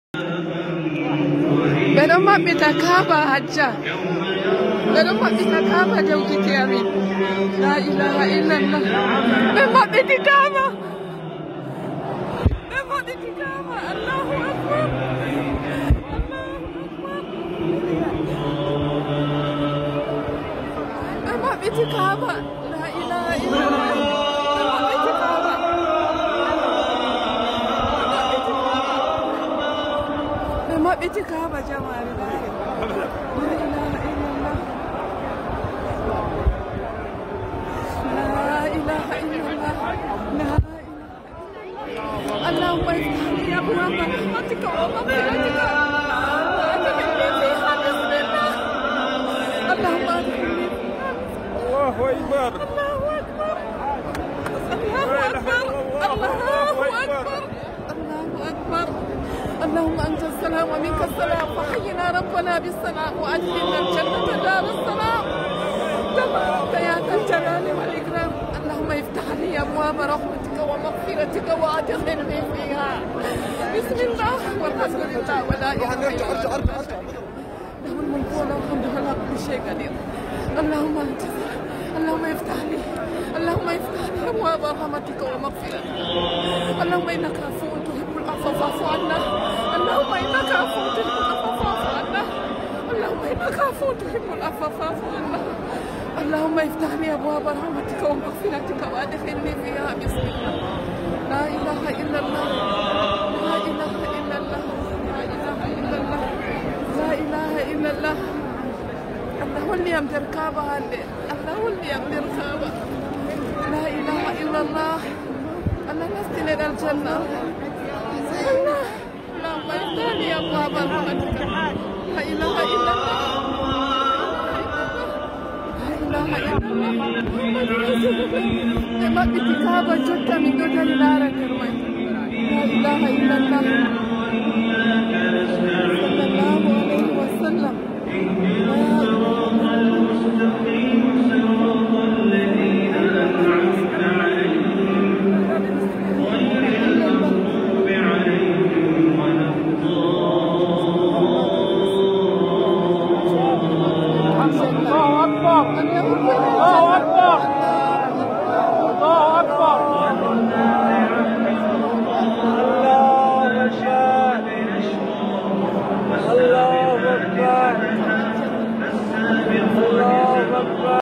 Discover Kabaa Beautiful Video | Tilawat-e-Quran | StoricalInfo
The Muslim call to prayer was heard and 2 rakaat prayers were observed while the imam and azan were present.
This happened on Friday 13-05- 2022